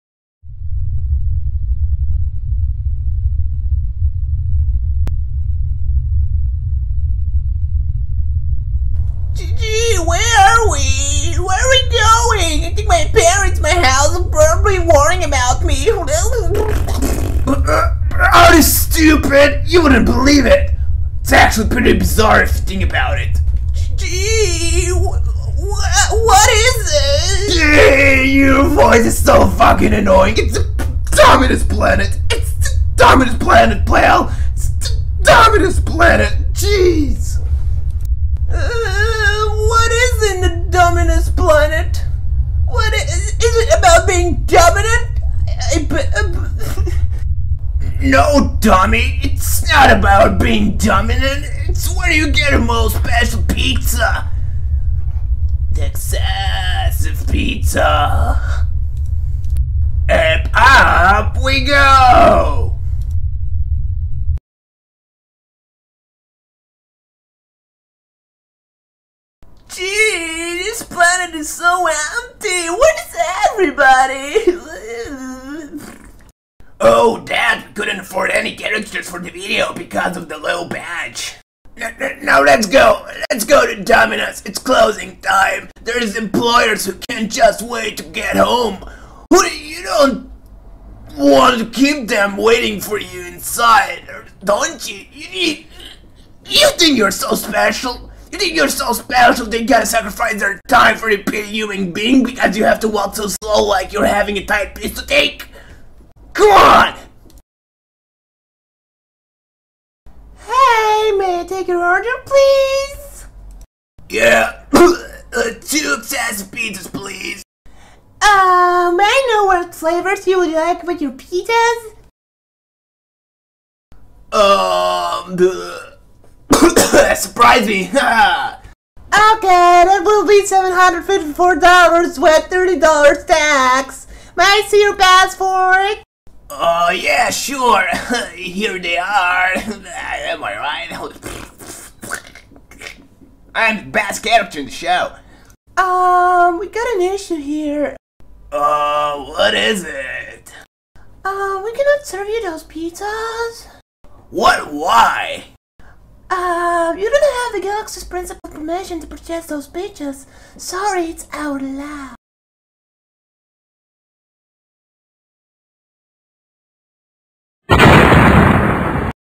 Domino's special pizza
end my life

My submission as a part of some Israeli brand Domino's pizza competition, I hope I choke on pizzas after I win